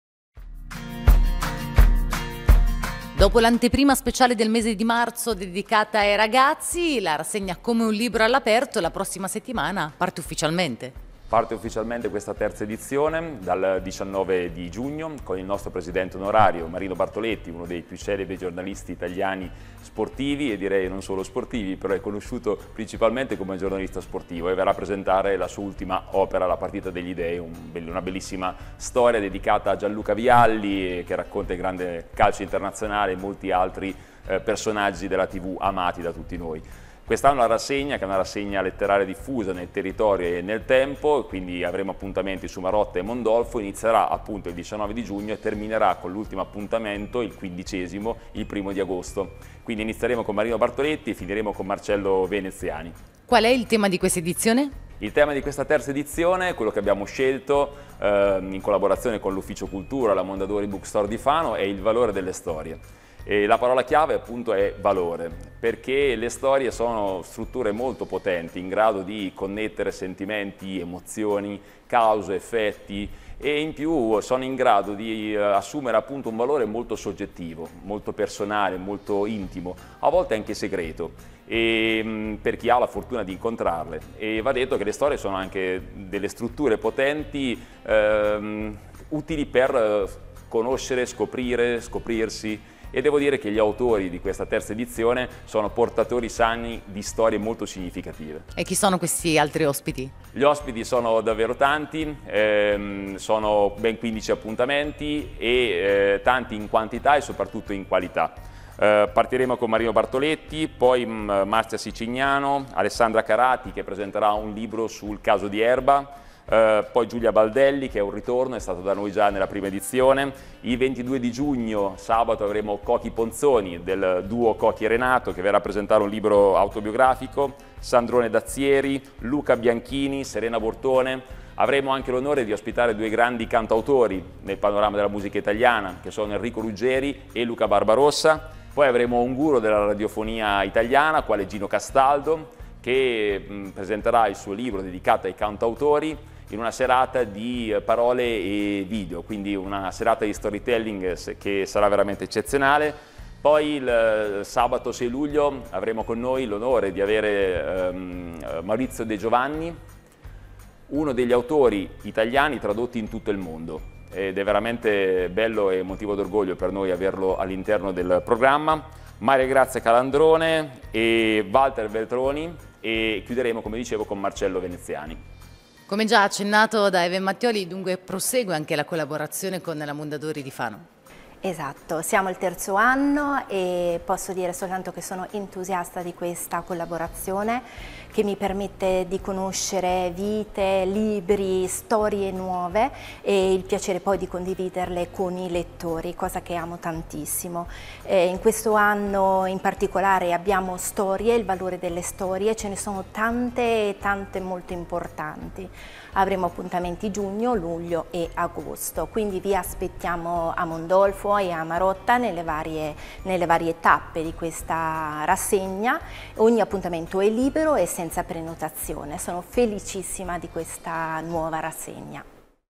Bartoletti da' il via a “Come un libro all'aperto”
Sarà Marino Bartoletti a dare il via, martedì 18 giugno a Mondolfo, alla rassegna letteraria “Come un libro all'aperto”. Sentiamo tutti i dettagli e gli ospiti della manifestazione nelle prossime interviste.